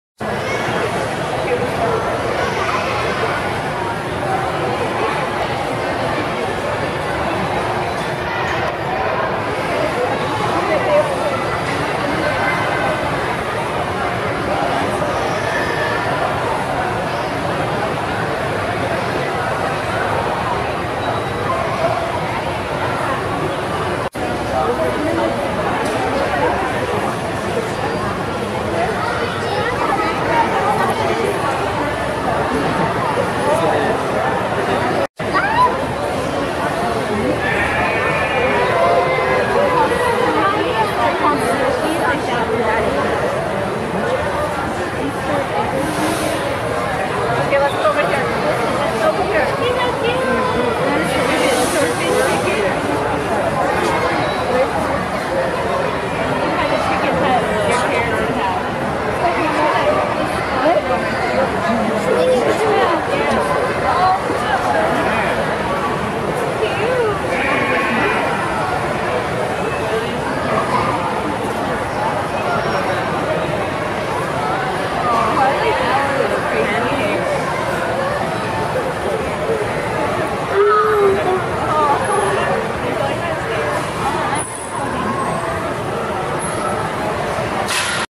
Ameraucana Easter Egger, Black Cochin Bantam, and Silkie Chickens
I've never seen these breeds of chicken before. These were at the San Diego County Fair in 2014. I couldn't get quiet eye level footage because of the fenced setting.